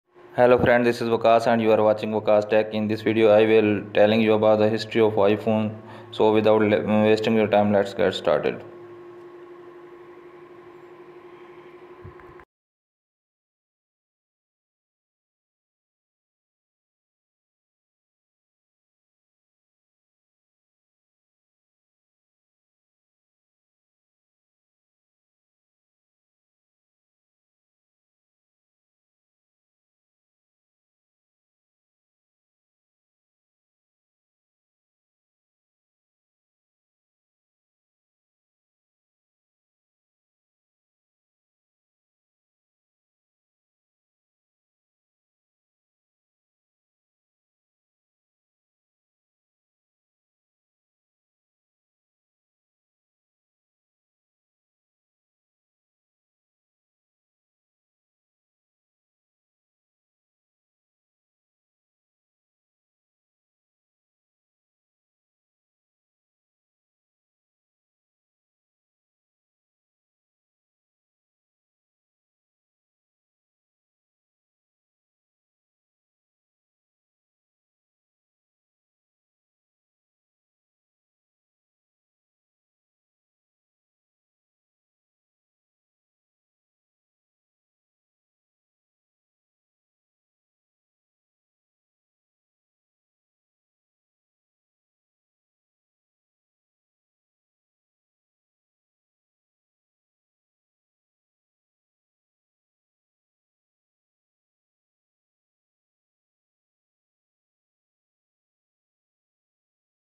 History of the iPhone//History of iPhone 2007 to 2020 Evolution
HI GUYS IN THIS VIDEO I AM SHOWING YOU ALL MODELS OF I PHONES

Watch the iPhone evolve through thirteen years of development. Starting with the original iPhone in 2007, which revolutionized the mobile phone industry, and ending with the second-generation iPhone SE in 2020. What's in store for the best selling smartphone in America,No one knows for sure. But it would appear that the iPhone will remain a leader in the smartphone industry for years to come.